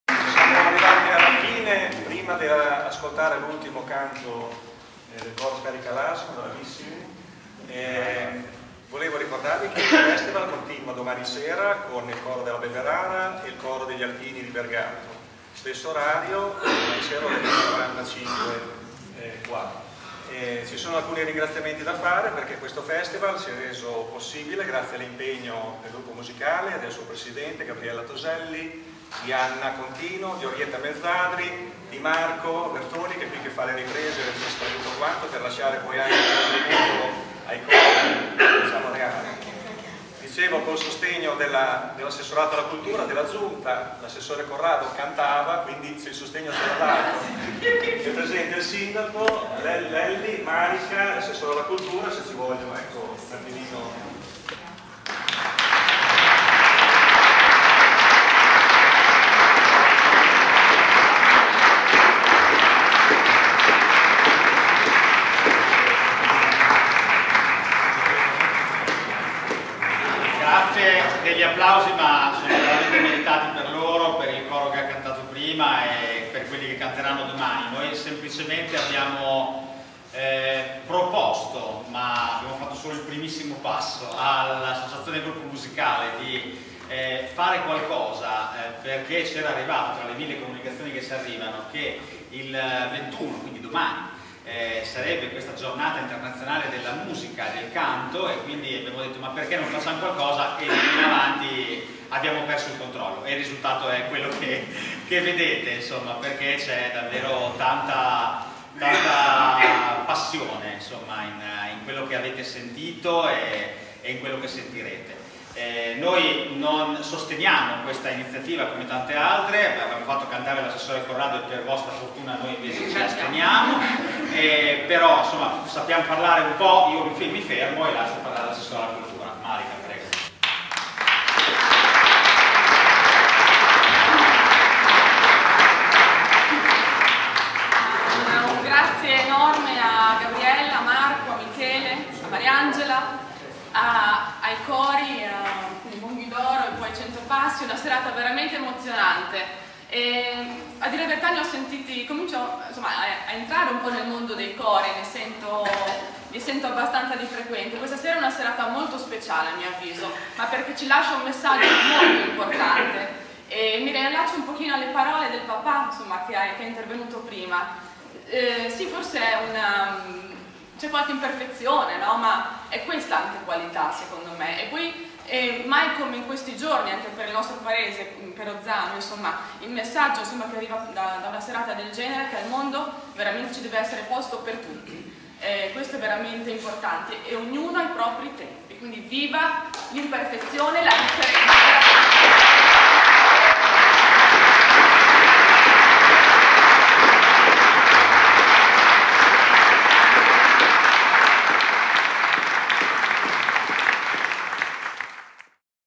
Ringraziamenti
Discorso del sindaco di Ozzano dell'Emilia Luca Lelli e l'assessore alla cultura Marika Cavina ad Ozzanoincanto 2017